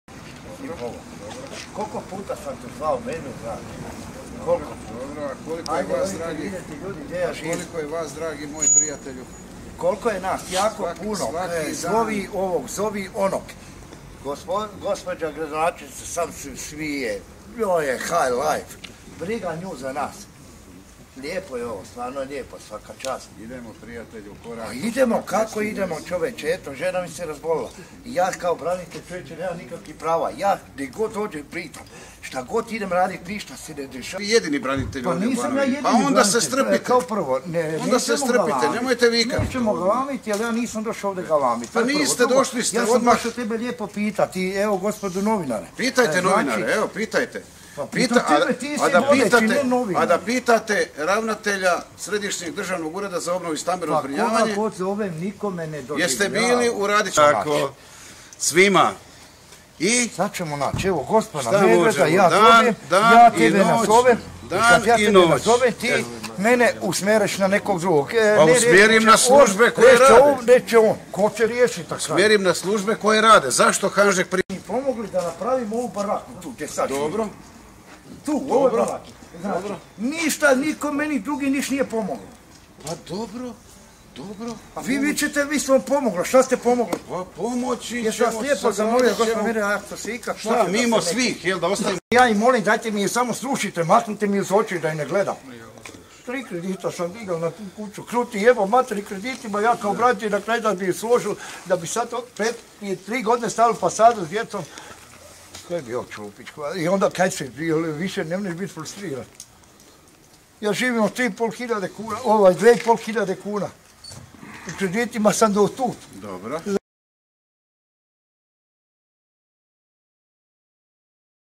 Žučna rasprava branitelja i Tome Medveda: "Vi vičete da ste mi pomogli, što ste mi pomogli?"
Verbalni sukob ministra Tome Medveda i branitelja Gordana nezadovoljnog brzinom obnove.